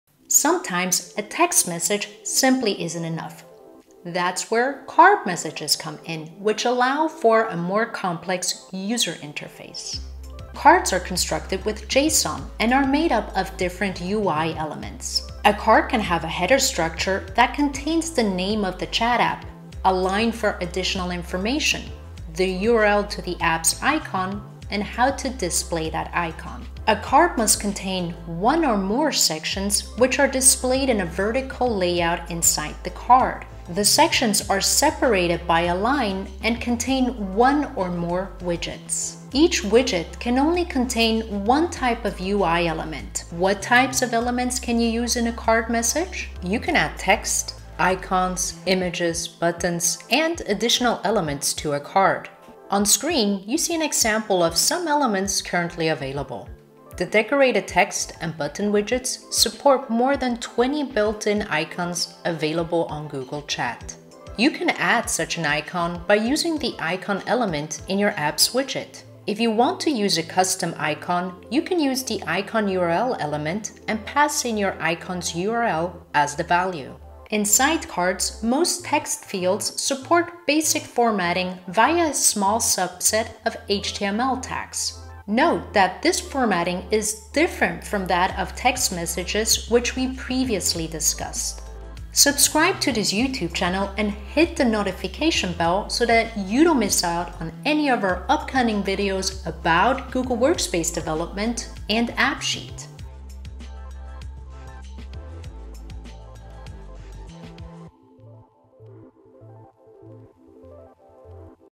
Google Chat apps - Card messages
In this video you will find out what card messages for custom Google Chat apps are and how to use them.